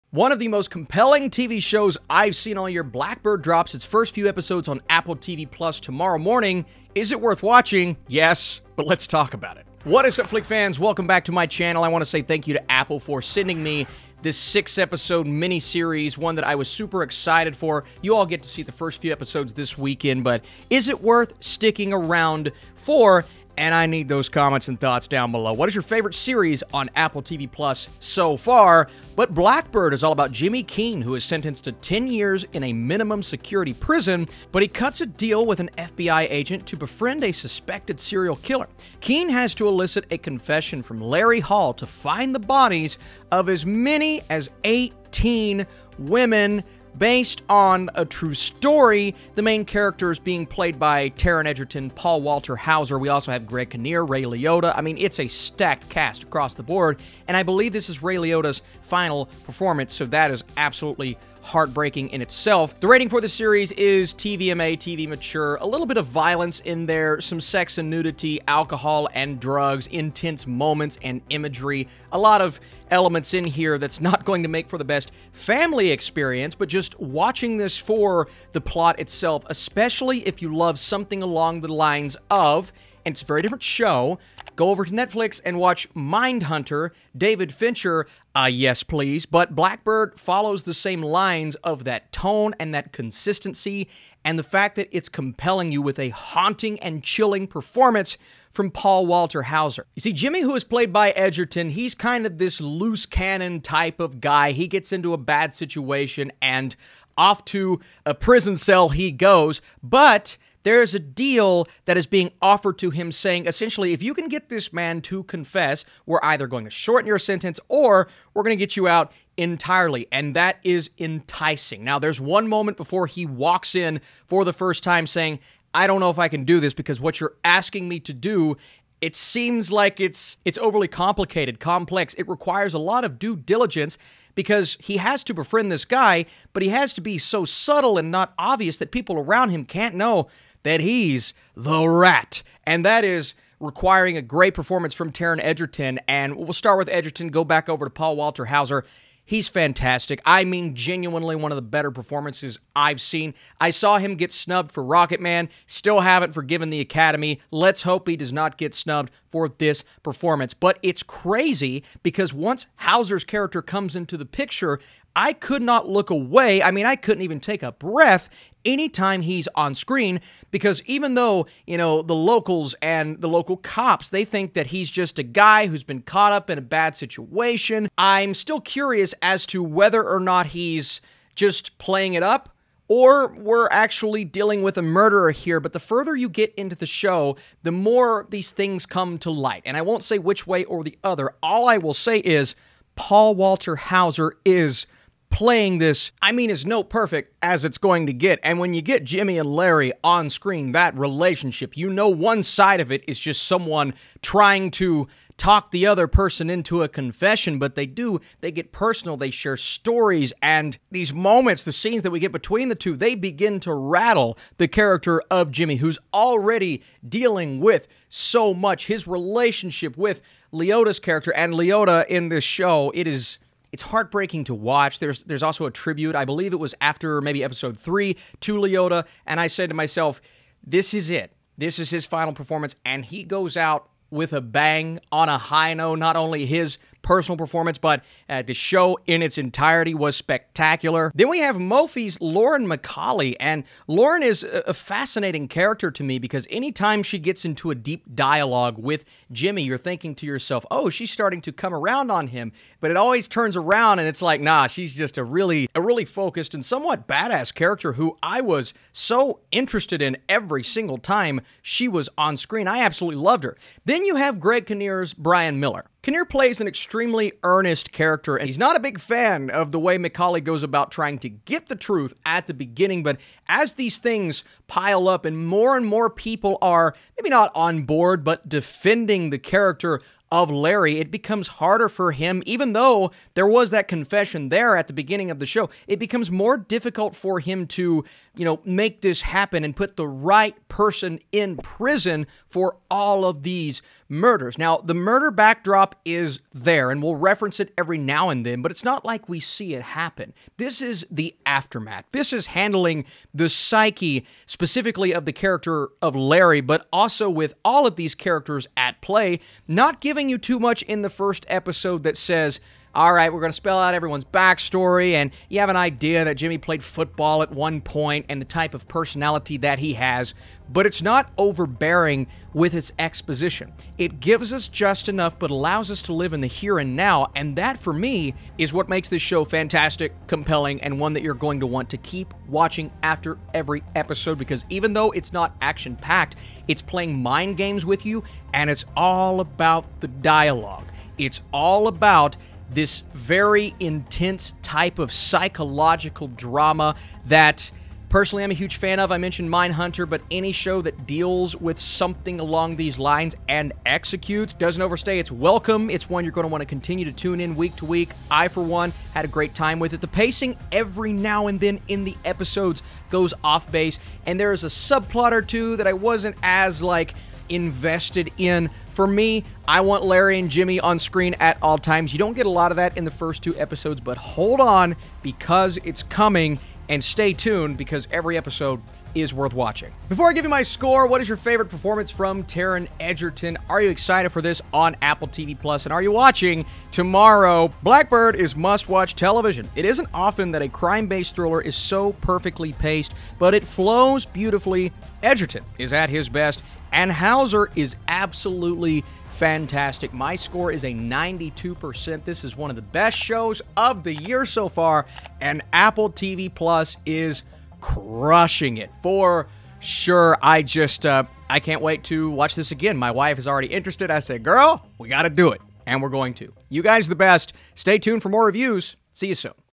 Black Bird is a THRILLING Series | Apple Review
This is my Black Bird Series Review. Black Bird is a Thrilling Series, and it is coming to Apple TV Plus the weekend. Leave your thoughts and Reaction to Black Bird, starring Taron Egerton and Paul Walter Hauser.
Jimmy Keene is sentenced to 10 years in a minimum security prison but he cuts a deal with the FBI to befriend a suspected serial killer. Keene has to elicit a confession from Larry Hall to find the bodies of as many as eighteen women. What is your Reaction and should I talk spoilers for episode 1 or 2?

black bird review
black bird apple review
black bird series review